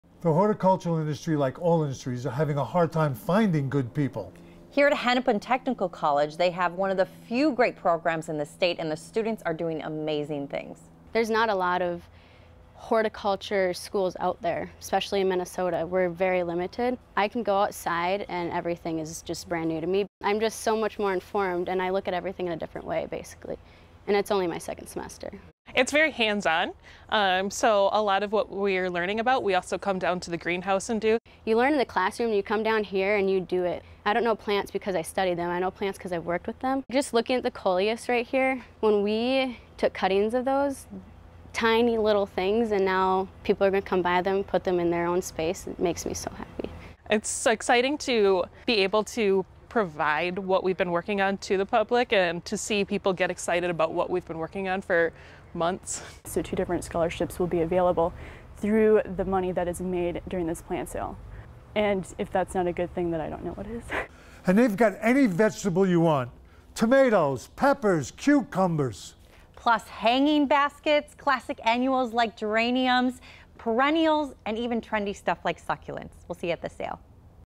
Grow with KARE: Hennepin Technical College Plant Sale
Hennepin Technical College have a horticulture program and the students there are doing amazing things.